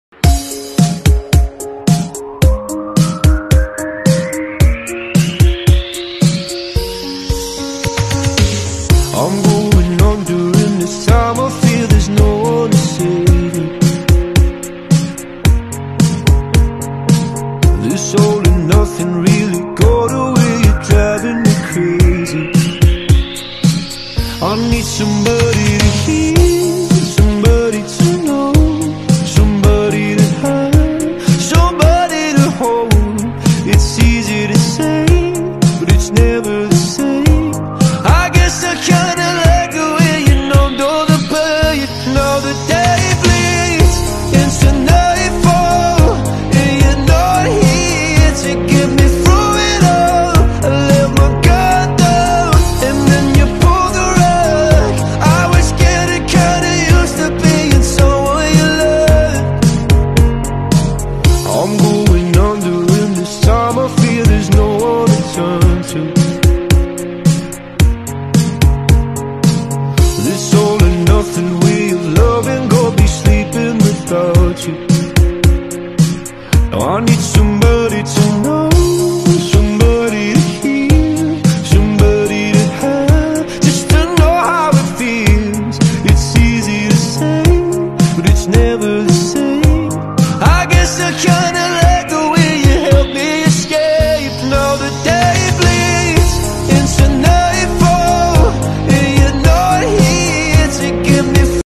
Multi-layer coating and laminating machine
This multi-layer lamination laminating machine can perform double-sided 7-layer extrusion lamination at one time. It is a low-cost liquid aseptic paper-plastic aluminum packaging material. Its packaging products include brick-shaped and pillow-shaped milk, juice, and tea drinks. The shelf life at room temperature is 2-12 months.